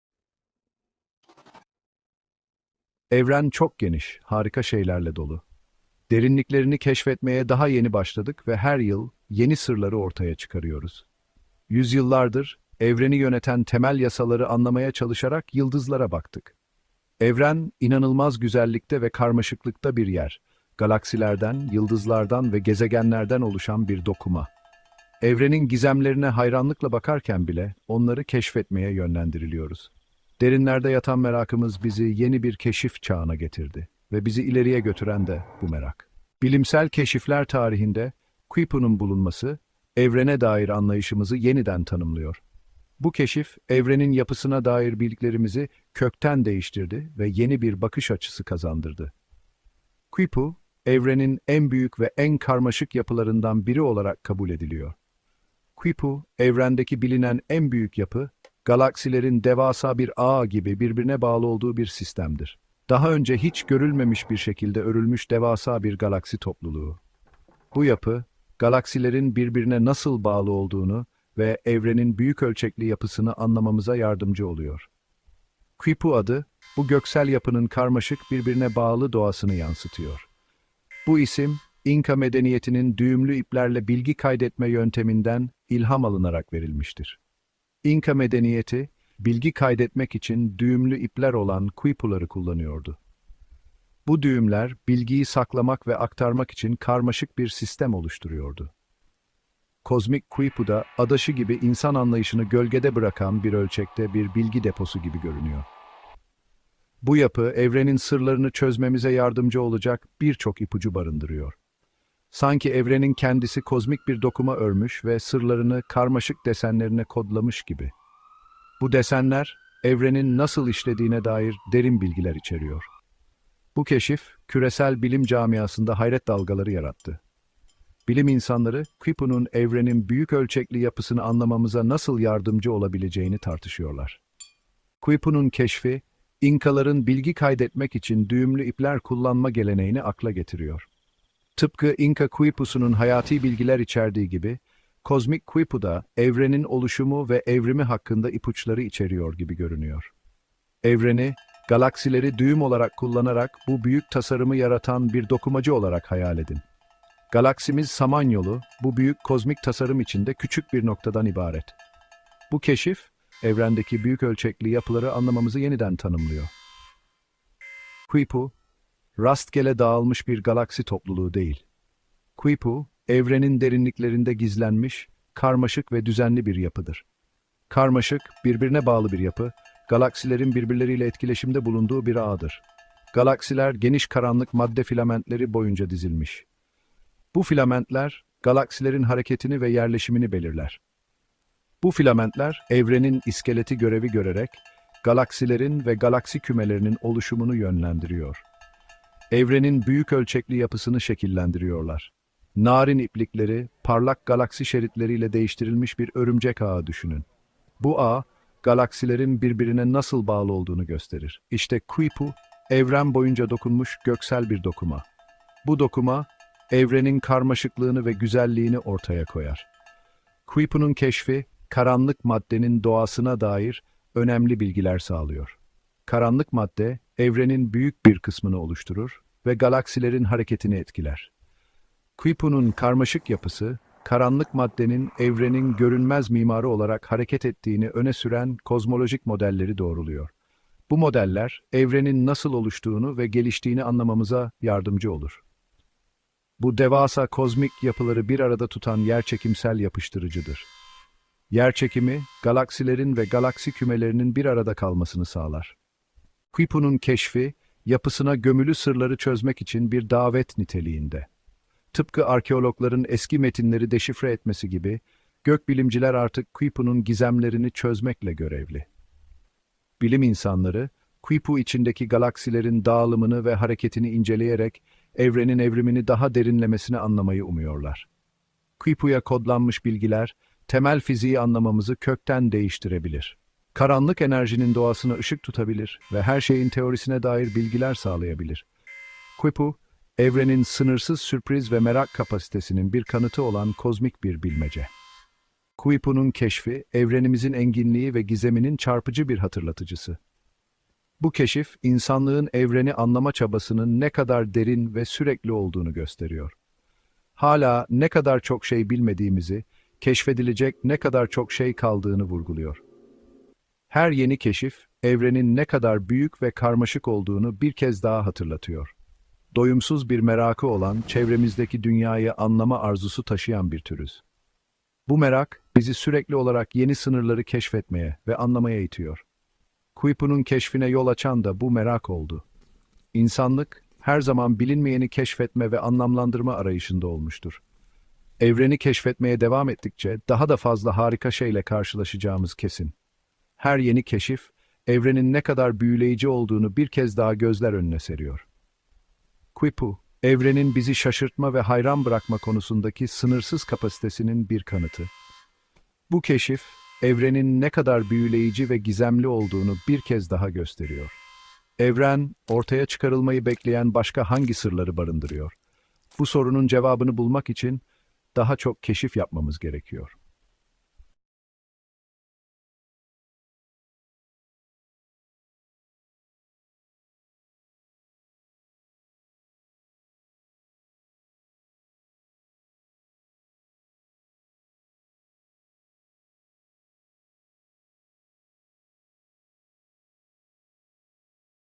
Evrenin En Büyük Sırrı: Quipu!
Evrenin en büyük sırrı ile karşınızdayız! 🌌 Bu etkileyici ve bilgilendirici konuşma ile Quipu'yu keşfedeceğiz. 1,3 milyar ışık yılı boyunca bu devasa yapı, galaksilerin nasıl bağlandığını ve evrenin büyük değişiminin anlaşılmasına yardımcı oluyor. İnka medeniyetinin düğümlü ip hesaplama sürecinden esinlenen Quipu, aslında evrenin karmaşıklığını gözlerin önüne seriyor. İnsanlık olarak bu sonsuzlukta ne kadar küçük çapta düşünürken, merakımızın bize aktarılan süreçlerle büyümeye hazır olun! 🚀

Bu heyecan verici ürünü paylaşmayı unutmayın! Yorumlarınızı bekliyoruz!

ANA HATLAR:

00:00:00 Gizemler Evreni
00:00:36 Quipu'nun Perde Arkası
00:02:14 Geçmişin Yankıları
00:02:48 Galaksilerden Oluşan Bir Goblen
00:04:16 Kozmik Kod
00:04:55 Harikalar Evreni